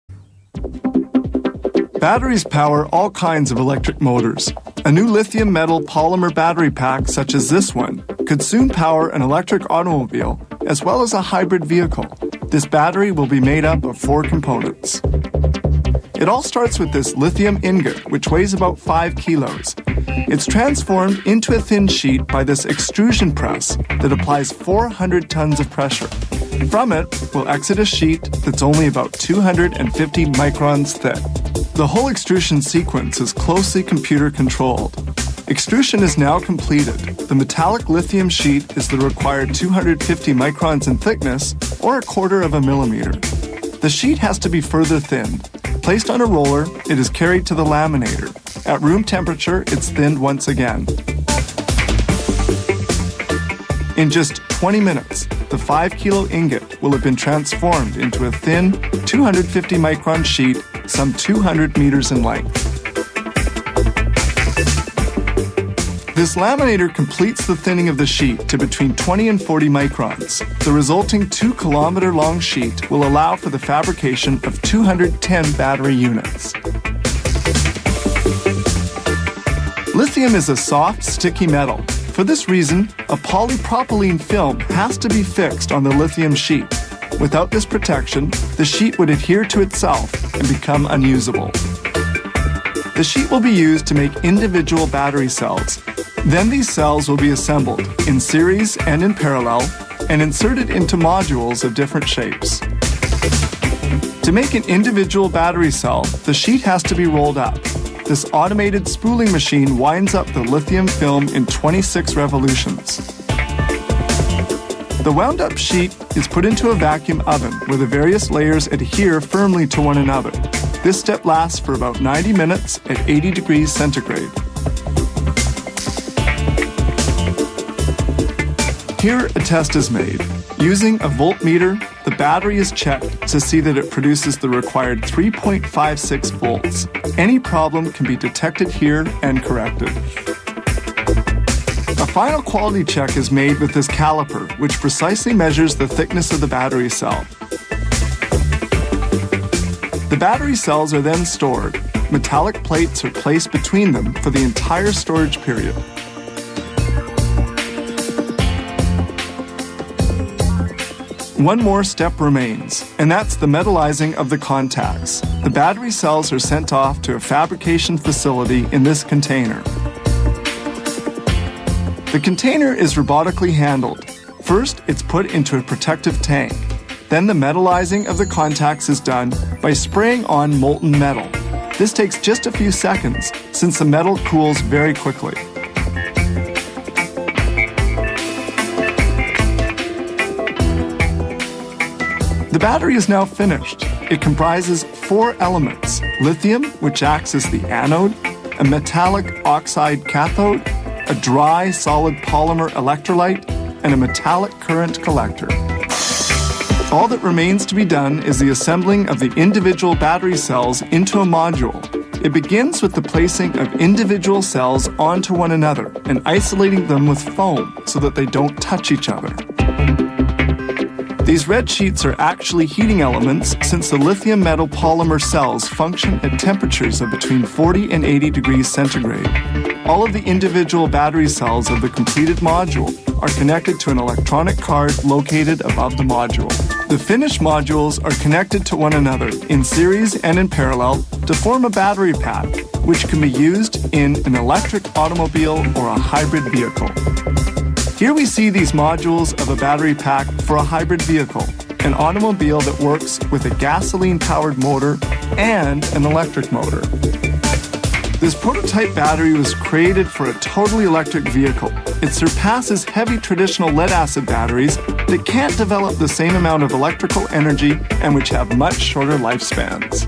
How to make Lithium Polymer Batteries for Electric Car
Really easy, isn't it? At least much more than manufacturing with sulfuric acid, or to make the chips from any mobile phone. Notice that Li-Po Batteries were designed to be cheaper and lighter that Li-Ion, now, Why can't I drive an EV with them?
(These are Lithium-Metal Polymer batteries, not Li-Ion)